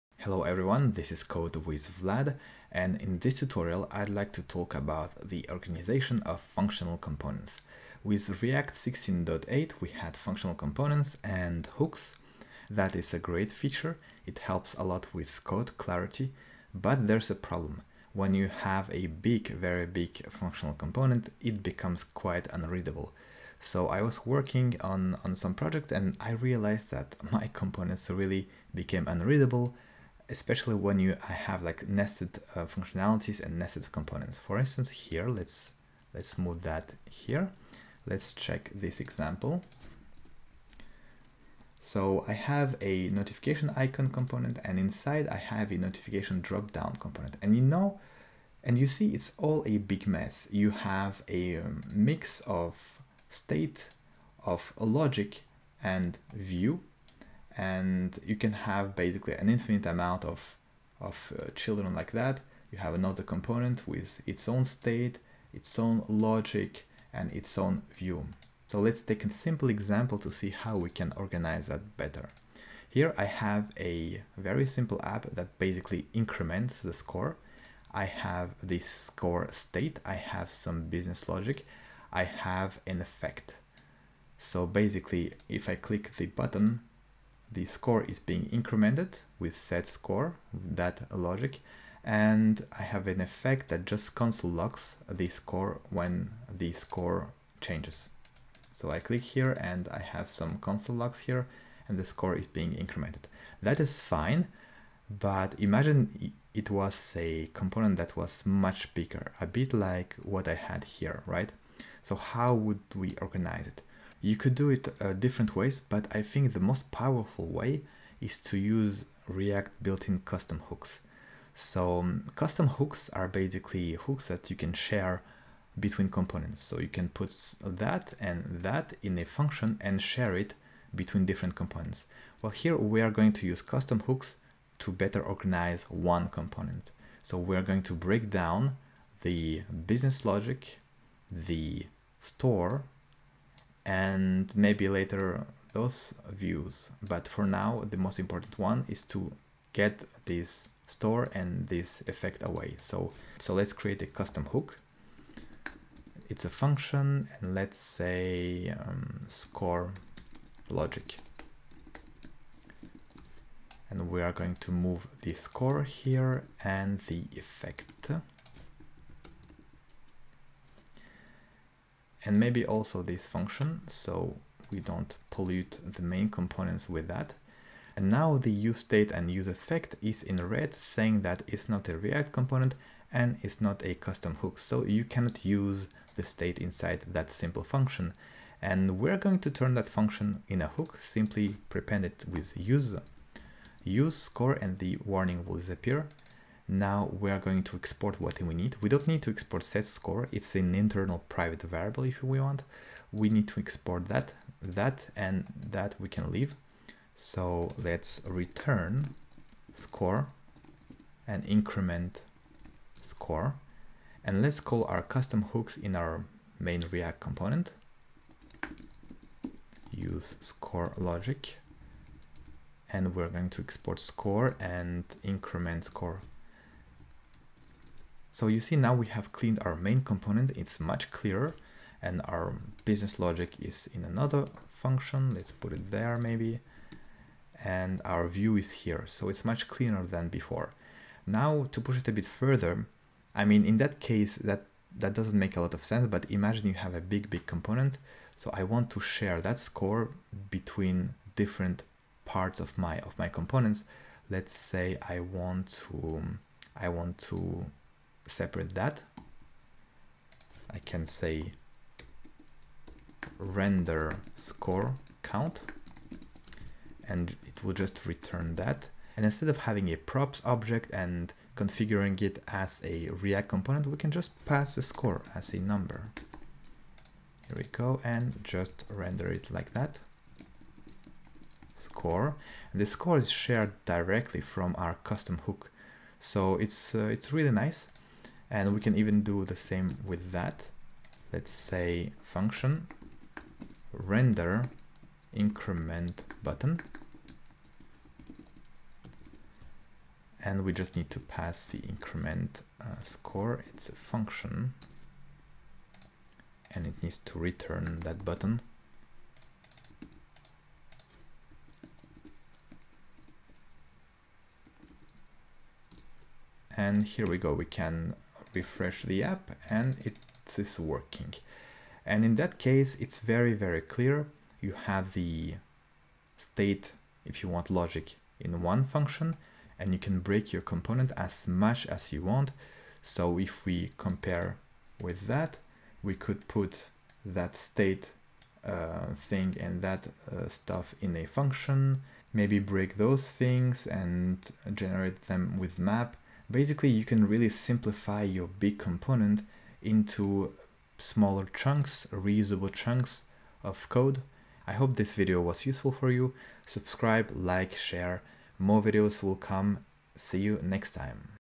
How to organise React Functional Components
React Functional Components and hooks are a great feature.
However, if we are not careful our components can become hard to maintain.

This video shows how to organise your functional components into smaller chunks using custom hooks.